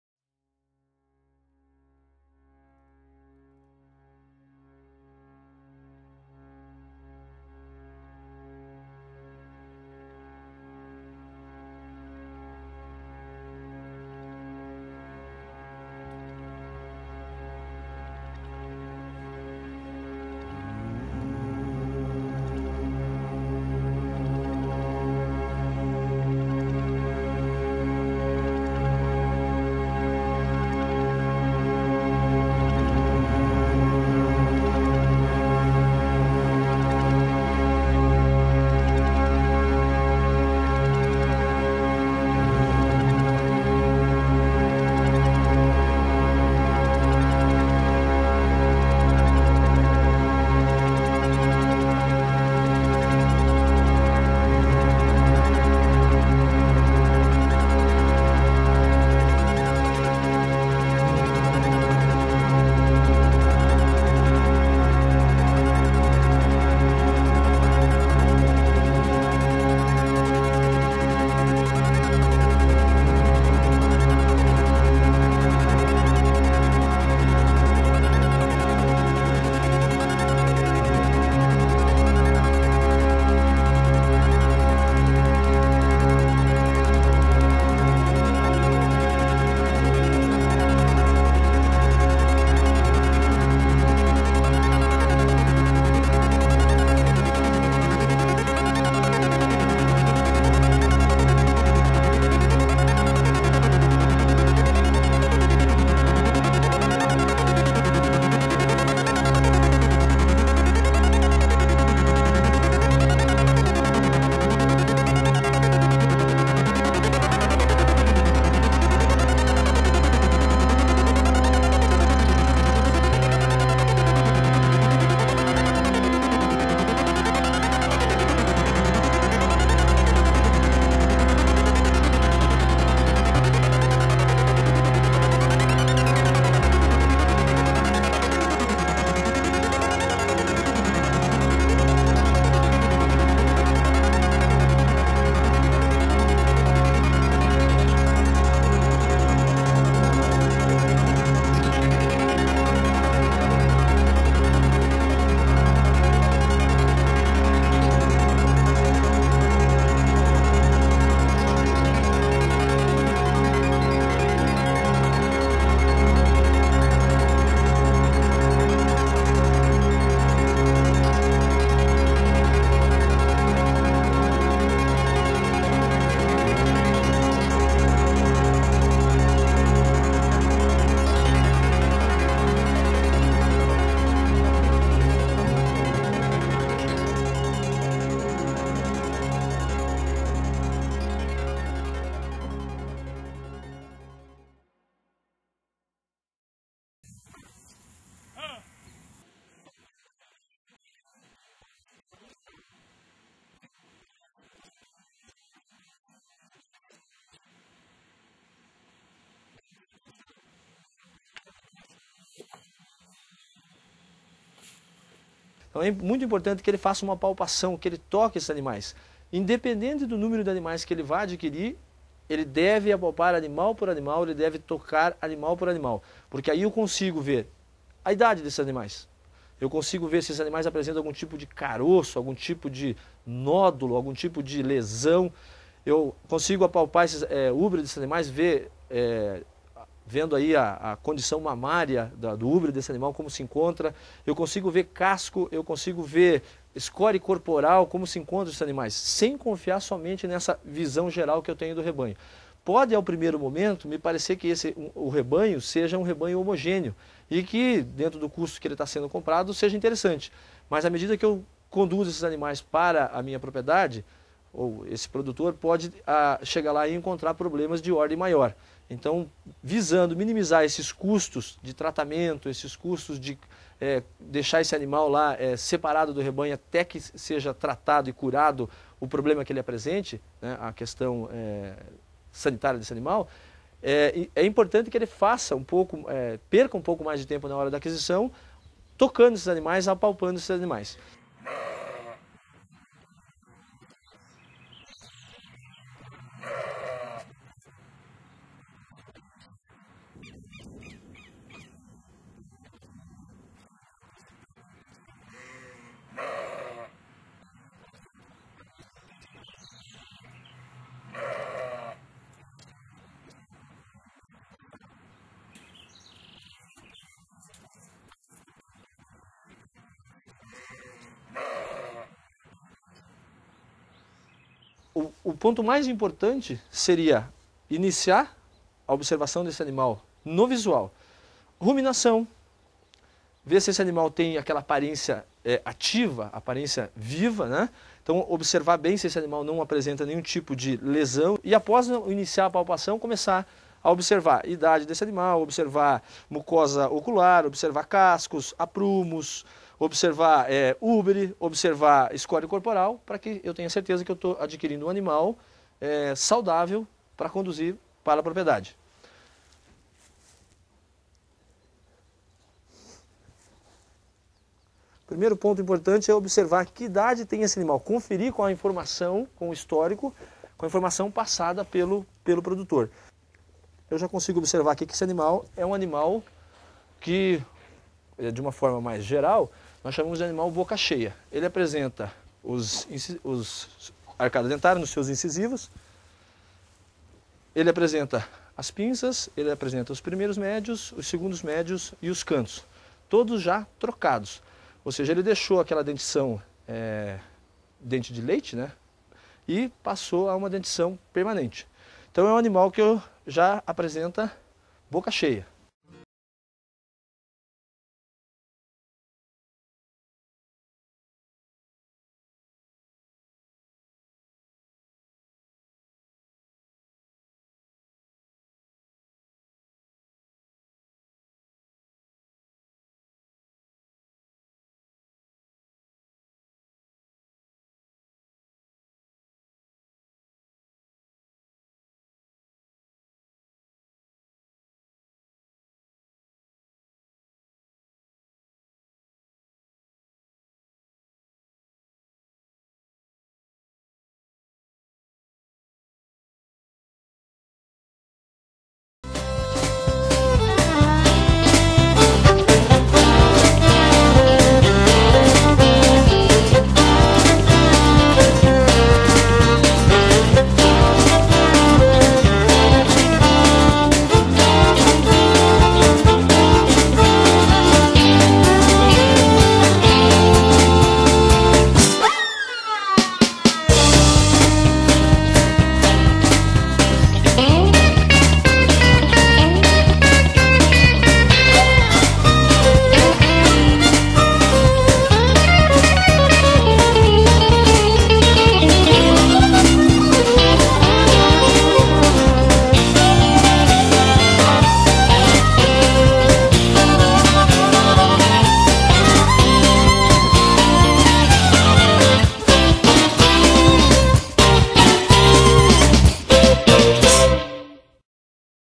Manejo Sanitário de Ovinos
Neste vídeo você vai saber as principais doenças dos ovinos.
Para a prevenção e controle de qualquer enfermidade, o melhor começo é conhecer os sinais de saúde e as medidas de higiene e profilaxia na criação.

Conteúdo :

Introdução
Sanidade Ovina
Medidas Preventivas
Doenças Metabólicas
Doenças Infecciosas
Doenças Parasitárias
Recomendação Final

Consultor: Zootecnista  Luiz Carlos de Matos